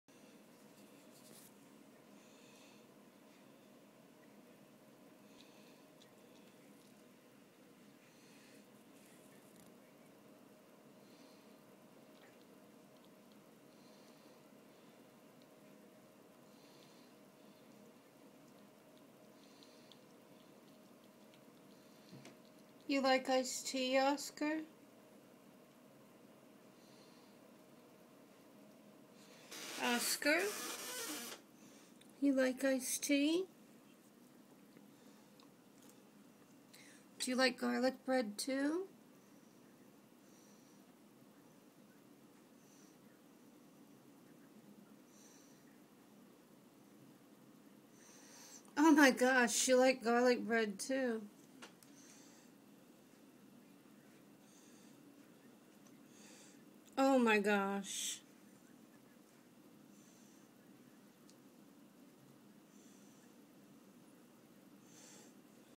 Cat drinking iced tea
My cat, Oscar, loves iced tea and garlic bread